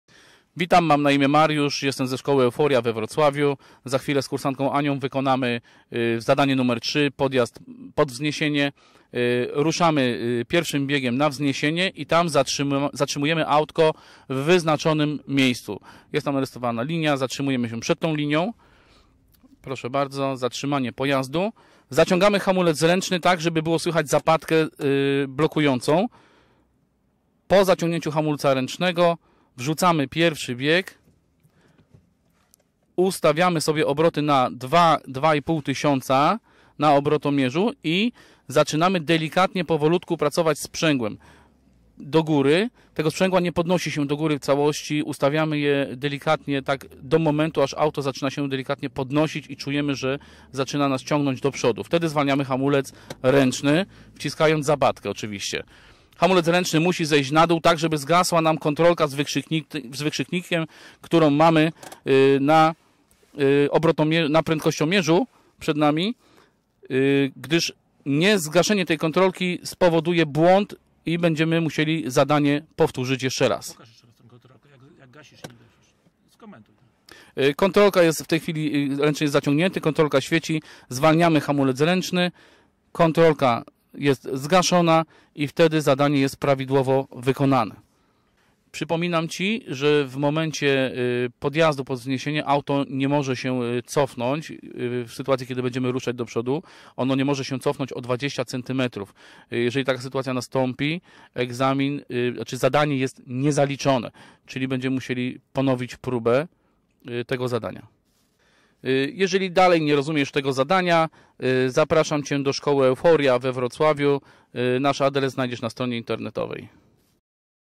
Zadanie egzaminacyjne górka: Wjazd pod wzniesienie na placu manewrowym
Przysłowiowa "górka" jest jednym z zadań egzaminacyjnych, na który możesz uwalić egzamin na placu manewrowym

Instruktor z OSK Euforia we Wrocławiu pokaże Ci prawidłowe wykonanie tego zadania egzaminacyjnego. Wykonuje je kursantka Ania, która w chwili obecnej jest już szczęśliwą posiadaczką prawa jazdy kategorii B.

Ośrodek Szkolenia Kierowców EUFORIADirections
Driving School
Address: Kniaziewicza 28/1A, Wrocław
Reviews
1 Google review